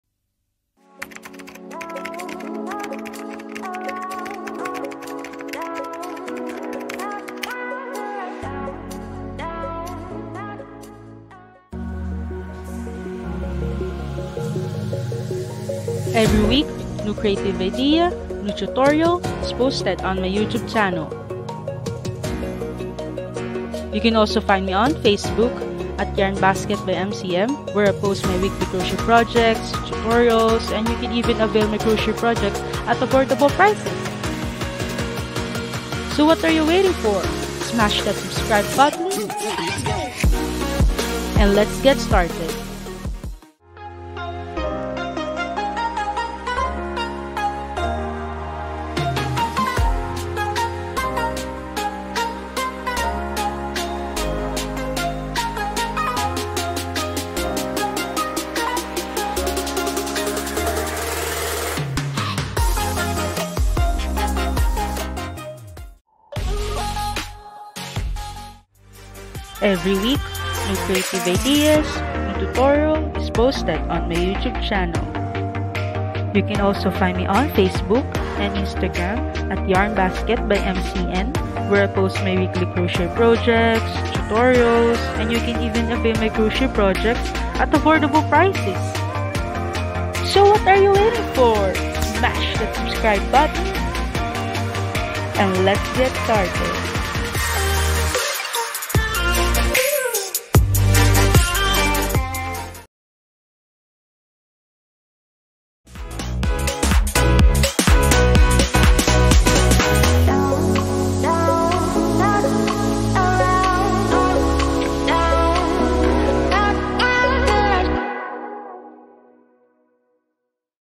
NEW INTRODUCTION & LOGO | Ms. Chris N.
Hey, crafties! It’s time for another crafternoon. This will be my new INTRODUCTION video!
Join me, and dare your mind your create!

I hope you enjoy this video and let me know what you think by commenting down below!

Happy crocheting!